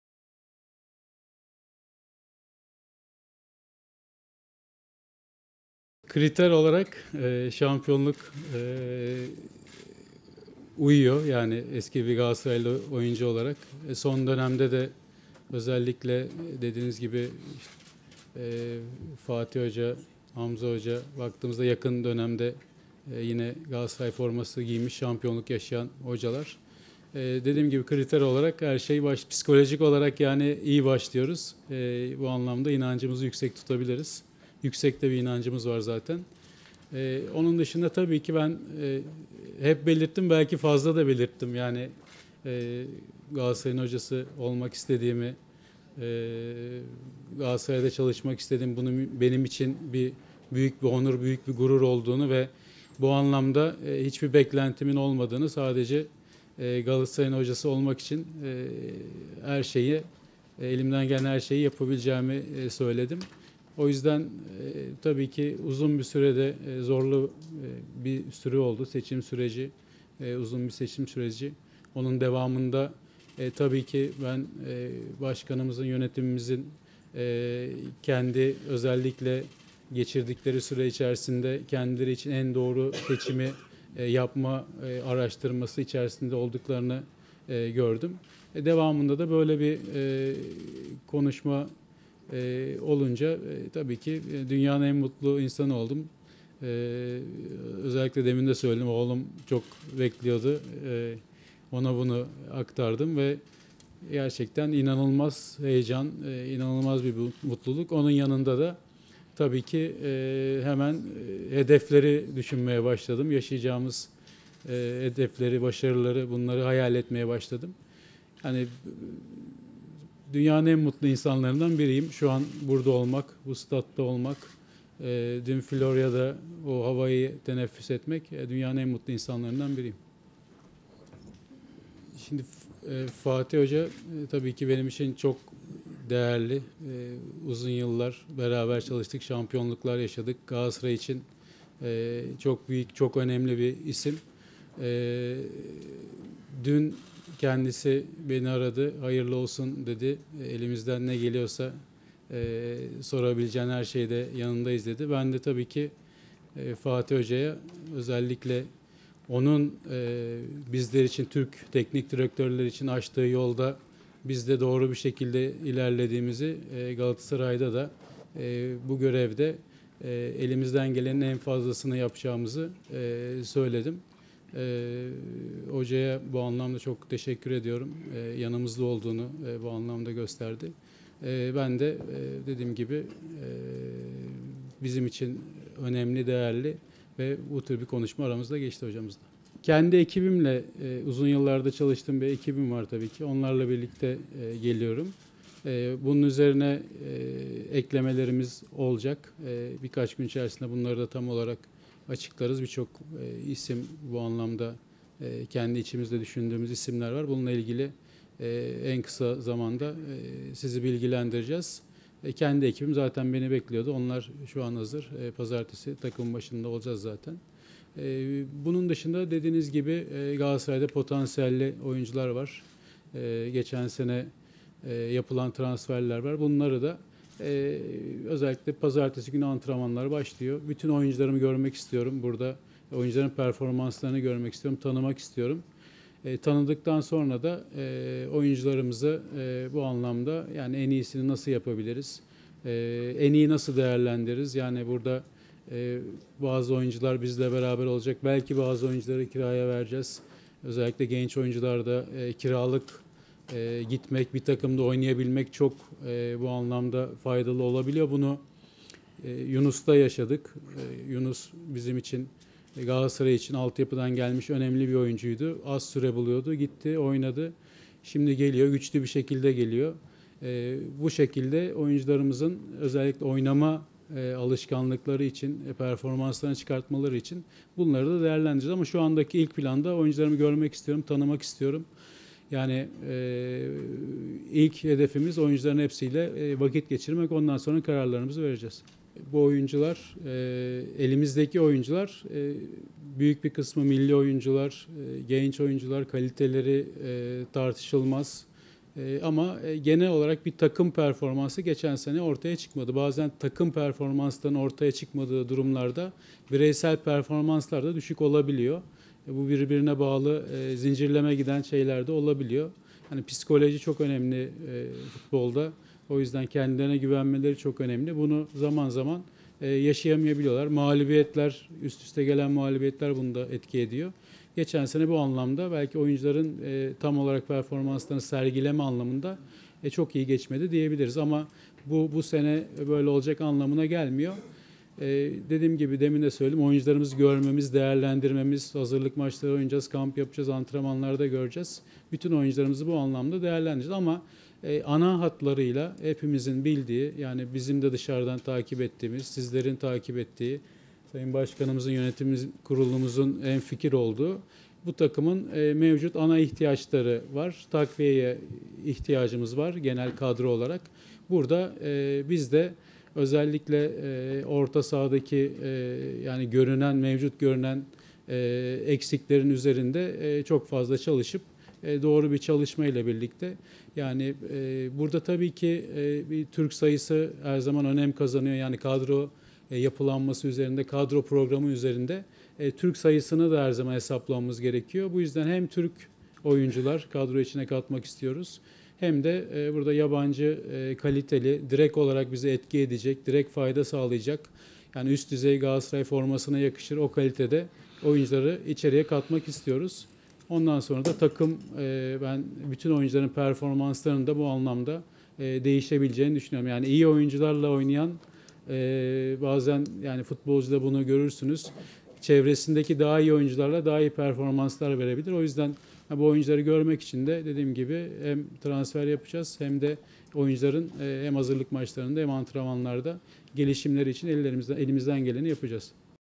Okan Buruk: Geçen sene yapılan transferler...
GÖRÜNTÜ DÖKÜMÜ :

- Galatasaray'ın yeni teknik direktörü Okan Buruk'un konuşması Galatasaray'ın yeni teknik direktörü Okan Buruk, yaşadığı mutluluğu anlattı:
- "Teklif gelince dünyanın en mutlu insanı oldum. İnanılmaz bir heyecan ve mutluluk var. Hemen hedefleri ve yaşayacağımız başarıları hayal etmeye başladım"
- "Fatih hoca çok değerli, uzun yıllar beraber çalıştık, şampiyonluklar yaşadık. Galatasaray için çok önemli bir isim. Kendisi dün beni aradı, 'Hayırlı olsun. Elimizden ne geliyorsa yanınızdayız.' dedi"
- "Kadroya hem Türk hem de bize doğrudan katkı sağlayacak, kaliteli, Galatasaray'a yakışır seviyede yabancı oyuncuları almak istiyoruz"
- "Galatasaray için buradayım. Başarıdan başka bir beklentim yok. Türkiye'nin en fazla şampiyon olan takımına bir şampiyonluk daha eklemeye çalışacağız"
İSTANBUL (AA) - Galatasaray Futbol Takımı'nda teknik direktörlüğe getirilen Okan Buruk, "Şu anda dünyanın en mutlu insanlarından biriyim." dedi.
Okan Buruk, Nef Stadı'nda başkan Dursun Özbek'in de katıldığı imza töreninin ardından basın mensuplarının sorularını yanıtladı.
Galatasaray'da şampiyonluk yaşayan Türk teknik direktörlerin daha önce sarı-kırmızılı formayı terlettiğinin hatırlatılması üzerine Buruk, "Yakın dönemde Fatih Terim ve Hamza Hamzaoğlu, Galatasaray forması giymiş, şampiyonluk yaşayan hocalar. Kriter olarak uyuyor. Psikolojik olarak iyi başlıyoruz. Yüksek bir inancımız var. Galatasaray'ın hocası olmak istediğimi belki fazla belirttim. Galatasaray'da çalışmak istediğimi, bunun benim için büyük bir onur ve gurur olduğunu, bir beklentimin olmadığını, sadece Galatasaray'ın hocası olmak için elimden gelen her şeyi yapabileceğimi söyledim. Uzun bir seçim süreci oldu. Devamında başkanımız ve yönetimimizin kendileri için en doğruyu yapma çalışması içinde olduklarını gördüm. Teklif gelince dünyanın en mutlu insanı oldum. İnanılmaz bir heyecan ve mutluluk var. Hemen hedefleri ve yaşayacağımız başarıları hayal etmeye başladım. Şu anda dünyanın en mutlu insanlarından biriyim." diye konuştu.

- "Fatih hoca yanımızda olduğunu gösterdi"

Okan Buruk, Galatasaray tarihinin en başarılı teknik direktörü olan Fatih Terim'in kendisini arayarak yanında olduğunu söylediğini belirtti.
Futbolculuk döneminde Fatih Terim ile çalışan Buruk, "Fatih hoca çok değerli, uzun yıllar beraber çalıştık, şampiyonluklar yaşadık. Galatasaray için çok önemli bir isim. Kendisi dün beni aradı, 'Hayırlı olsun. Elimizden ne geliyorsa yanınızdayız.' dedi. Onun Türk teknik direktörler için açtığı yolda ilerlediğimizi, bu görevde elimizden gelenin fazlasını yapacağımızı söyledim. Hocaya çok teşekkür ediyorum. Yanımızda olduğunu gösterdi." ifadelerini kullandı.
Uzun yıllardır çalıştığı ekibinin kendisiyle olacağını aktaran Buruk, "Uzun yıllardır çalıştığım ekibimle geliyorum. Onun üzerine eklemelerimiz olacak. Birkaç gün içinde bunları açıklarız. Kendi ekibim hazır ve pazartesi günü takımın başında olacağız." değerlendirmesinde bulundu.

- "Galatasaray'da potansiyelli oyuncular var"

Sarı-kırmızılı takımın yeni teknik direktörü, öncelikli hedefinin mevcut kadrodaki oyuncuları tanımak olduğunu söyledi.
Kiradan dönen Yunus Akgün'ün yaşadığı gelişime vurgu yapan Okan Buruk, sözlerini şöyle sürdürdü:
"Galatasaray'da potansiyelli oyuncular var. Geçen sezon yapılan transferler var. Özellikle pazartesi günü bütün oyuncularımı görmek, performanslarına bakmak ve onları tanımak istiyorum. Sonrasında da oyuncularımızı en iyi nasıl değerlendireceğimize bakacağız. Bazıları bizimle devam edecek, bazılarını kiraya vereceğiz. Özellikle genç oyuncular için kiralık olarak bir takımda oynamak çok faydalı olabiliyor. Bunu Yunus Akgün'de yaşadık. Yunus, Galatasaray için altyapıdan gelmiş önemli bir oyuncuydu, az süre buluyordu. Gitti, oynadı ve güçlü bir şekilde geliyor. Bu şekilde futbolcularımızın oynama alışkanlıkları ve performanslarını yükseltmek için değerlendireceğiz ama ilk hedefimiz oyuncuların hepsiyle vakit geçirmek. Ondan sonra kararlarımızı vereceğiz."

- "Takviyeye ihtiyacımız var"

Buruk, Galatasaray'ın Süper Lig'i 13. bitirdiği geçen sezon bir takım performansı sergileyemediğini belirterek bazı bölgelere takviyeye ihtiyaç duyduklarını söyledi.
Sarı-kırmızılı takımdaki futbolcuların kaliteli olduğunu bildiren 48 yaşındaki teknik adam, şunları kaydetti:
"Elimizdeki oyuncuların büyük kısmı milli, genç ve kaliteleri tartışılmaz. Ancak geçen sezon genel olarak bir takım performansı ortaya çıkmadı. Bazen böyle durumlarda bireysel performanslar da düşük olabiliyor. Futbolda psikoloji ve kendilerine güvenmeleri çok önemli. Üst üste gelen mağlubiyetler bunda etkili oluyor. Geçen sezon oyuncular adına performanslarını sergileme anlamında iyi geçmedi. Ancak bu, bu sezon da böyle olacağı anlamına gelmiyor. Oyuncularımızı görüp değerlendirmemiz lazım. Ancak herkesin bildiği gibi bu takımın ana ihtiyaçları var. Takviyeye ihtiyacımız var. Orta sahadaki görünen eksiklerin üzerinde çalışacağız. Türk oyuncu sayısı da kadro yapılanması içinde çok önemli. Kadroya hem Türk h